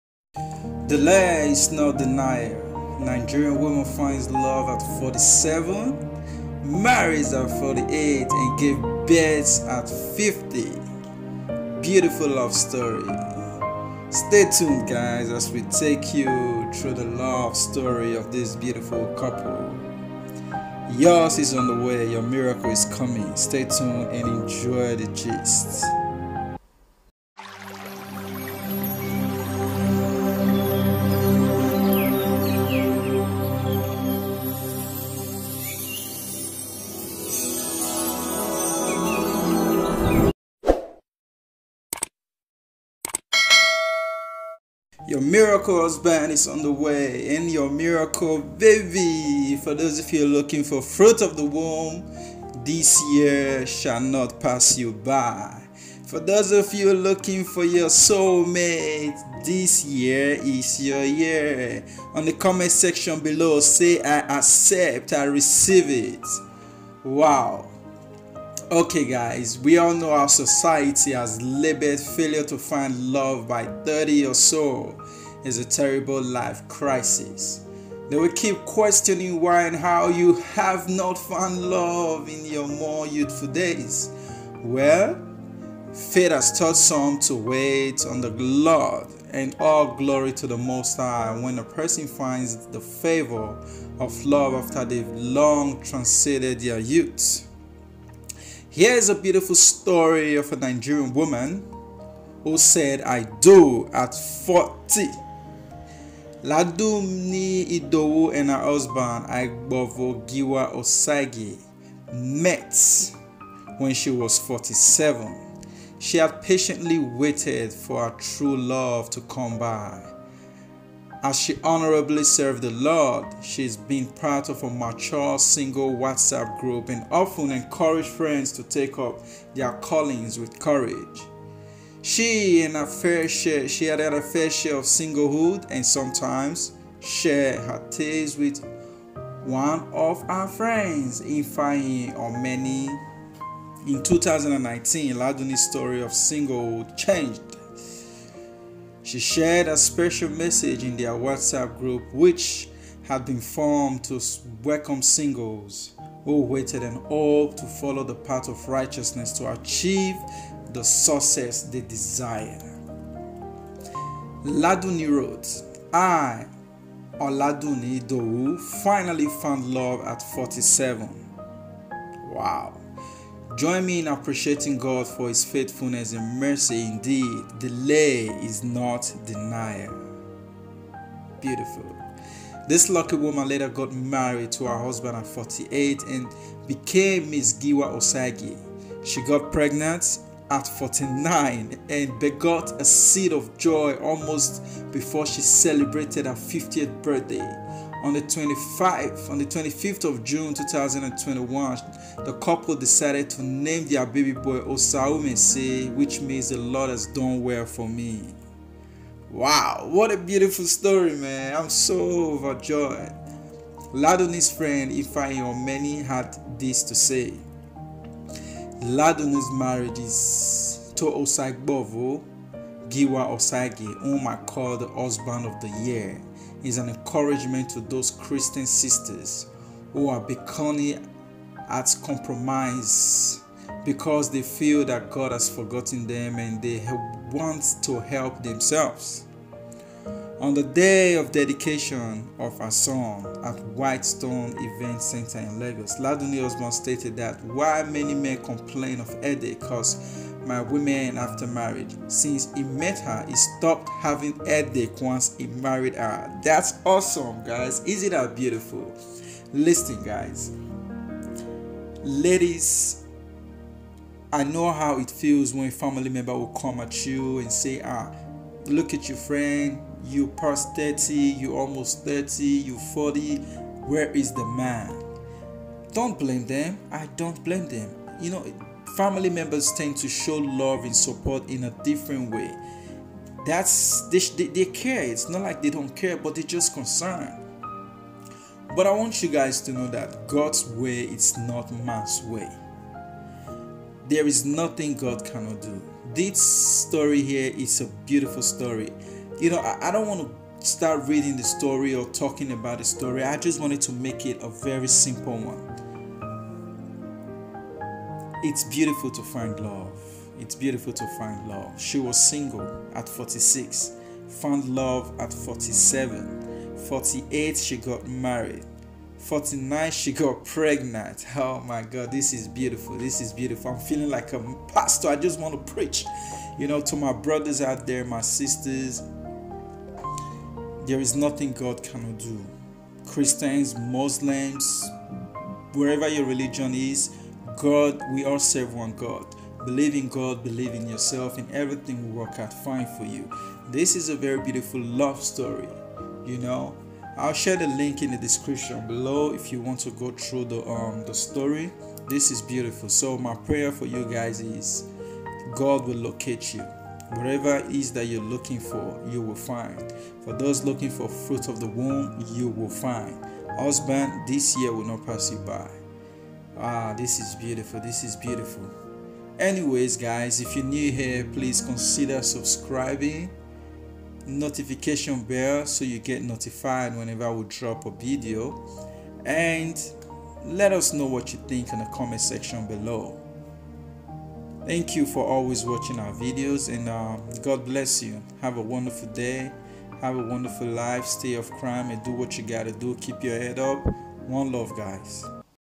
Nigerian woman found love at 47, married at 48 and gave birth to a baby boy before her 50th birthday
Welcome to the home of GIST. Everything Entertainment, OGBONGE TORI and Many more